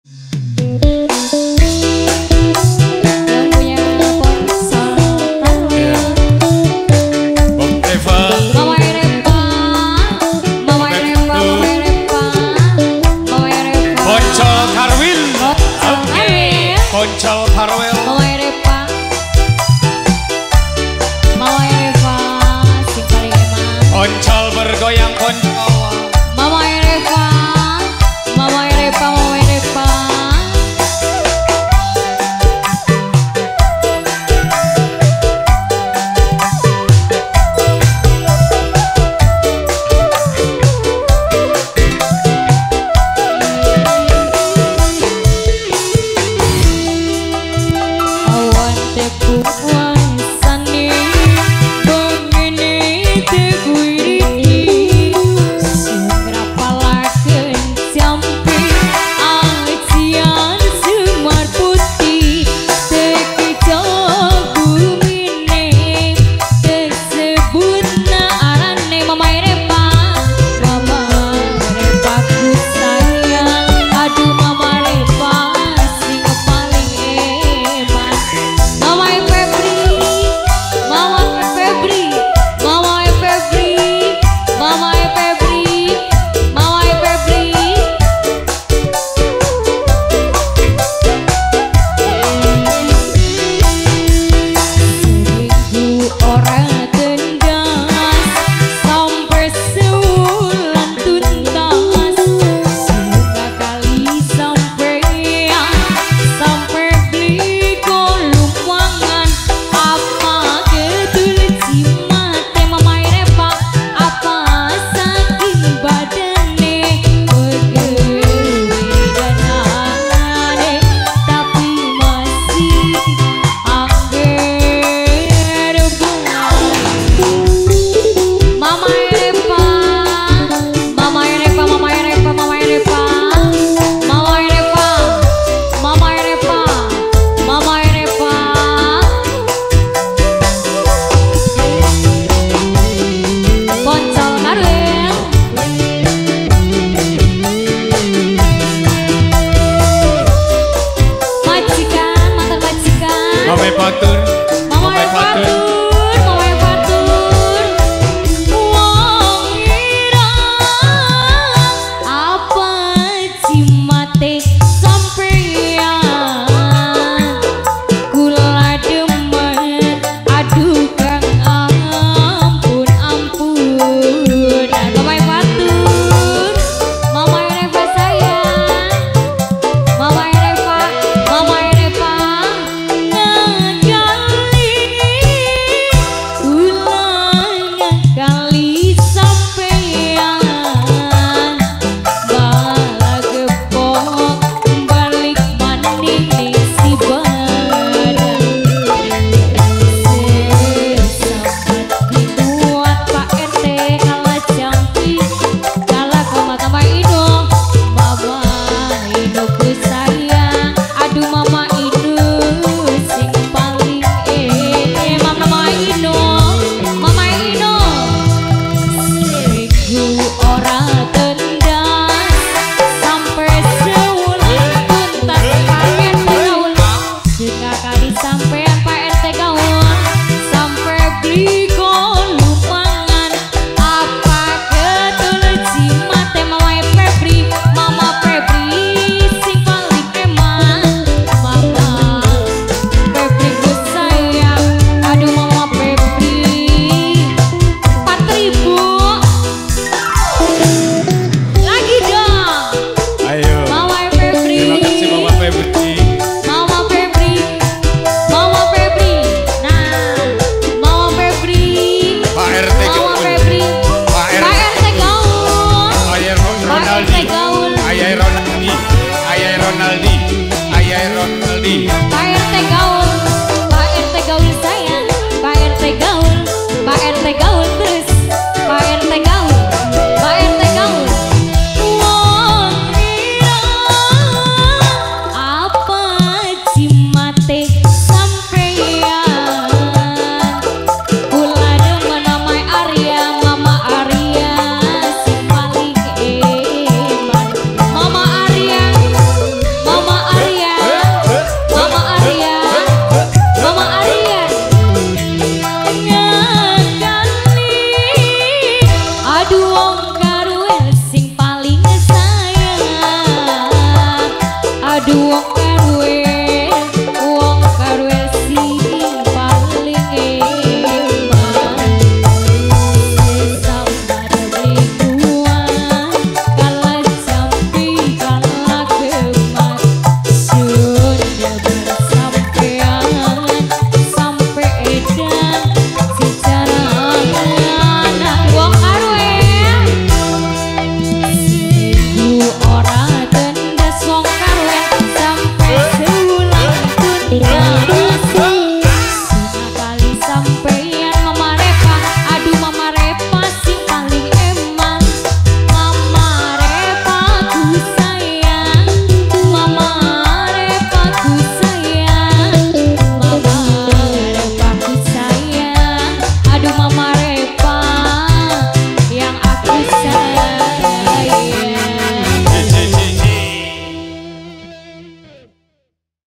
25 WONG IRENG - LIVE STREAM||BUROK MA NADA||RUNGKANG-LOSARI-BREBES-JUM'AT,17 MEI 2024
KHITANAN    : VIDAL AL FARREL { VIDAL }
PUTRA DARI  : BPK. KAMSARI { AYI } & IBU. ROHAYATI { YATI }
PELAKSANAAN : JUM'AT, 17 MEI 2024
TEMPAT      : DESA. RUNGKANG - KEC. LOSARI - KAB. BREBES - PROV. JAWATENGAH